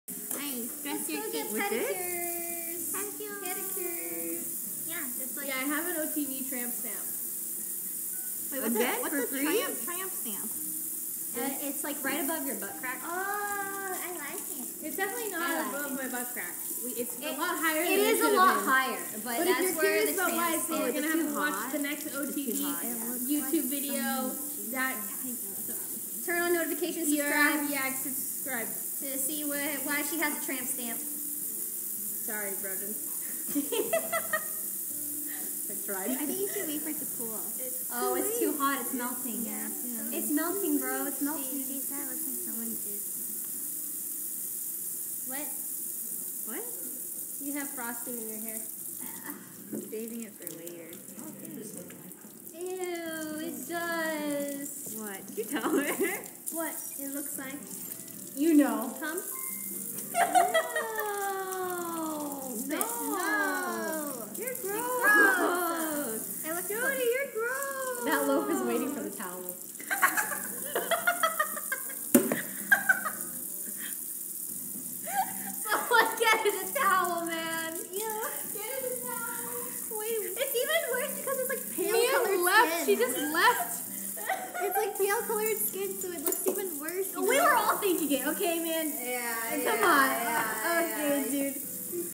Miyoung Straight-Up LEFT After Jodi & Rae Talk About C*M
Miyoung Straight-Up LEFT After Jodi & Rae Talk About C*M ft. iGumdrop